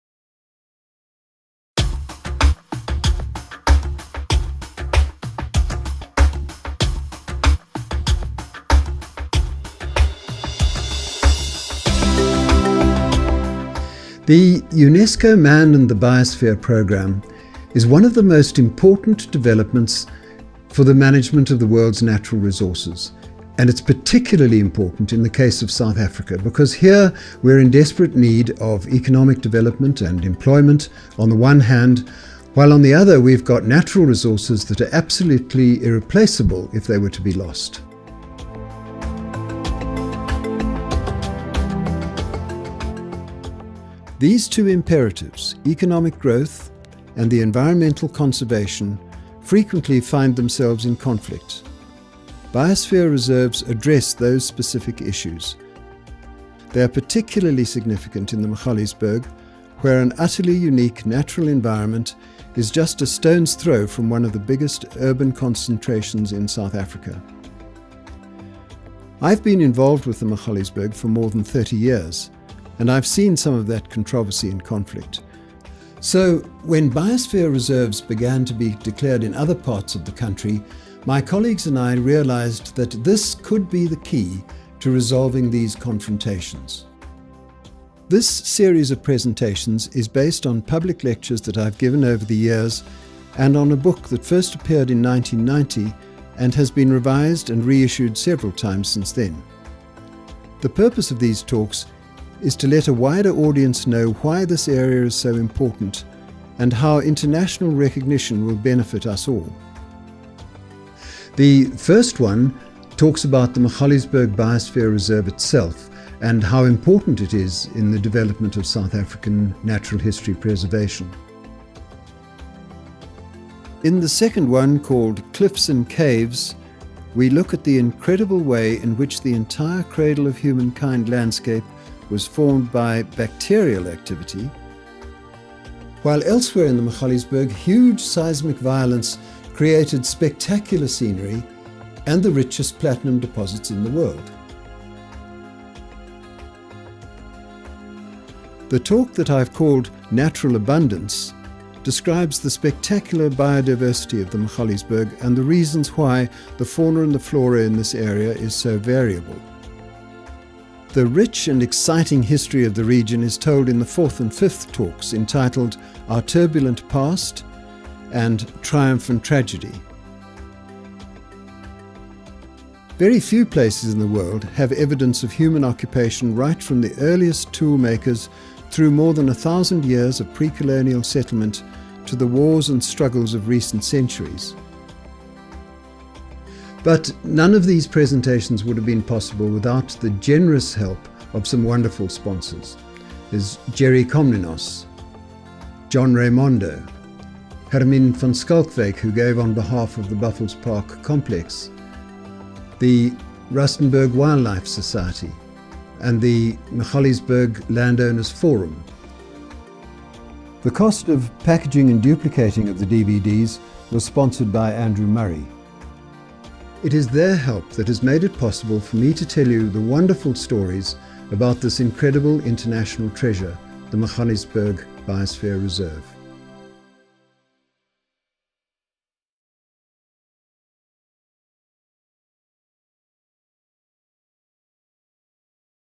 Introduction to the Magaliesberg - Part 1
The series presents a wealth of information on one of the most interesting regions of South Africa. The Magaliesberg Biosphere Reserve, that includes the Cradle of Humankind, was proclaimed by UNESCO in 2015. Biosphere Reserves strive to combine economic development with ecosystem preservation.

Introduction to the Magaliesberg gives the background to why the series was recorded.